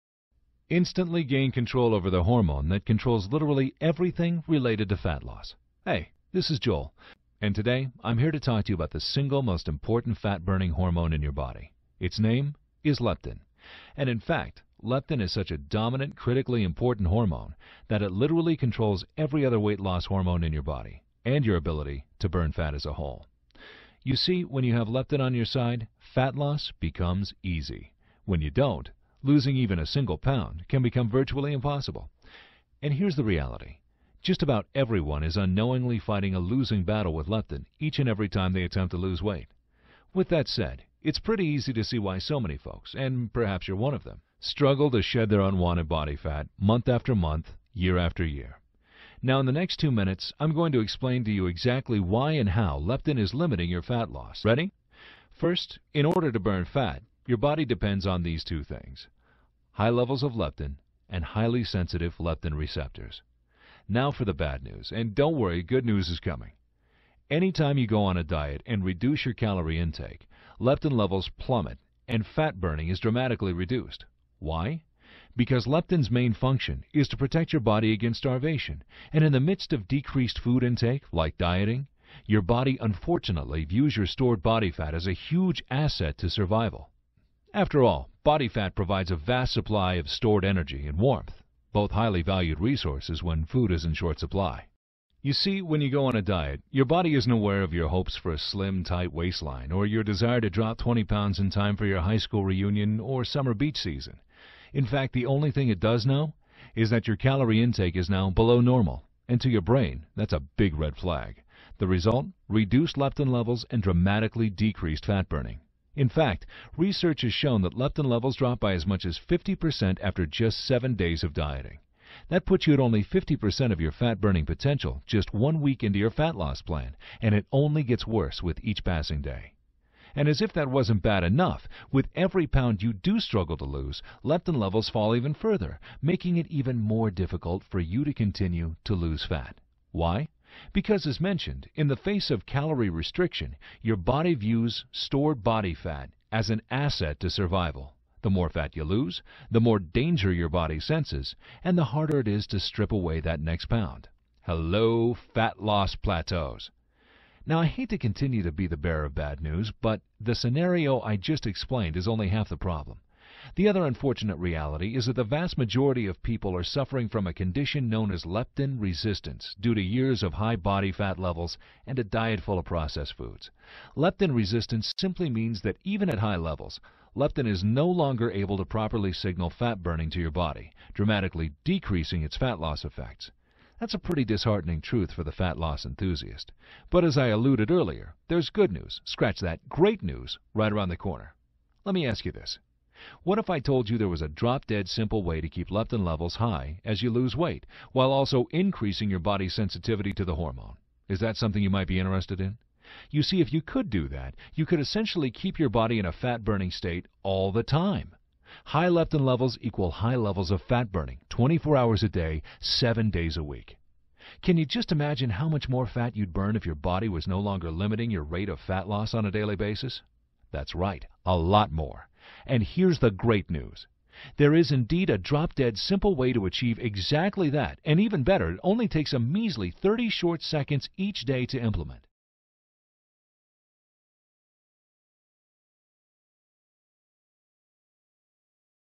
leptin2
Leptin cartoon explains how fatburning works